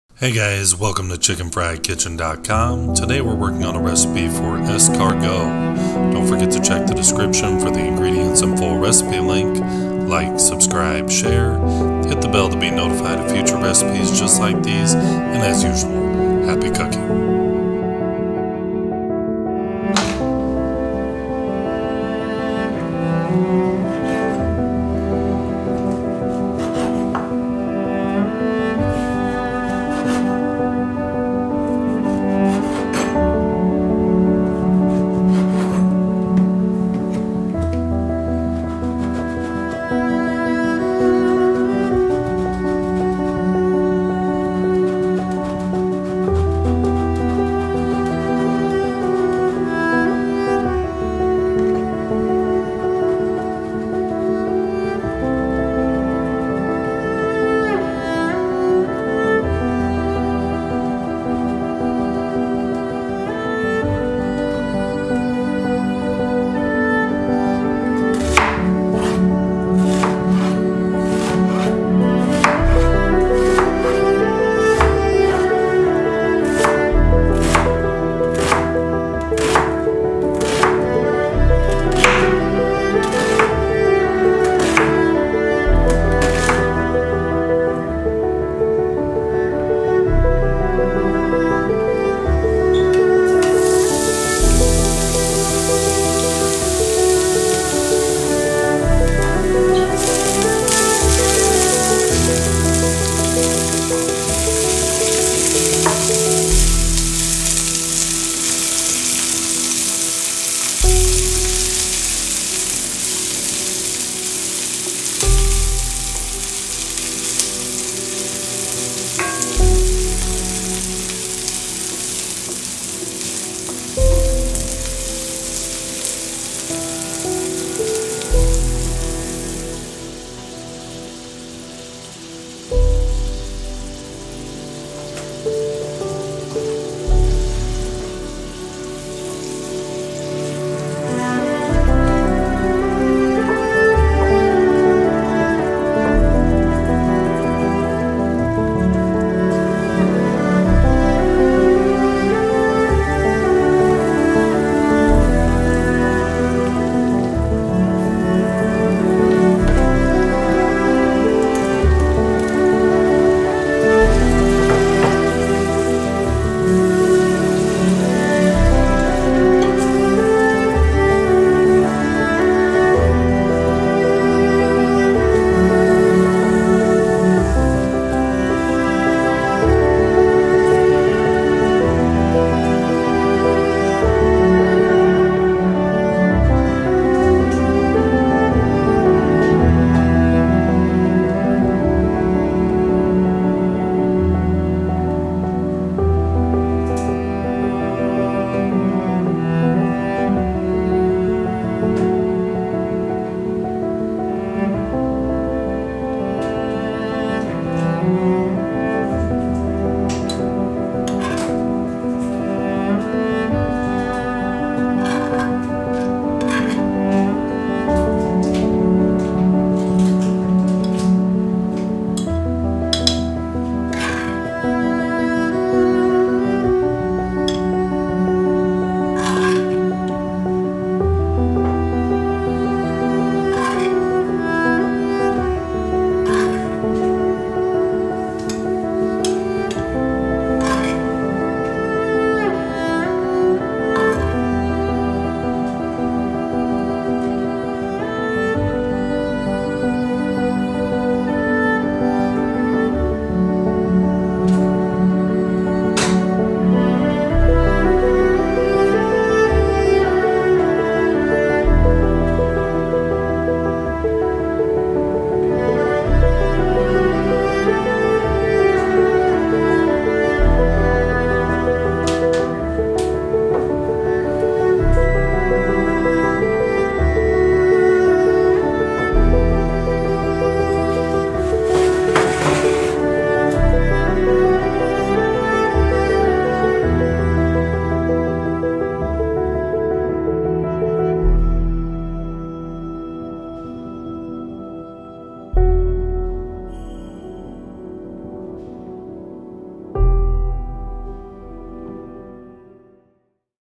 Escargot
Escargot is a French delicacy and comfort food at the same time. If you've never tried it or made it, I highly recommend you try this recipe before even going to a restaurant to have it. You'll quickly find that homemade escargot is better than most establishments.

CAST IRON

BAKING DISHES

INGREDIENTS
2 Tbsp Extra Virgin Olive Oil

1⁄2 Red Onion – diced

1⁄4 cup Chardonnay

1 Stick Salted Butter

1 Tbsp Minced Garlic

3 Tbsp Fresh Italian Parsley – chopped

1 Tbsp Fresh Rosemary – chopped

1⁄2 tsp Kosher Salt

1⁄2 tsp Black Pepper

1⁄2 tsp Garlic Powder

7.5 Oz Very Large Wild Burgundy Snails – drained and rinsed

1⁄4 cup Havarti cheese – shredded

1⁄4 cup Gruyere Cheese – shredded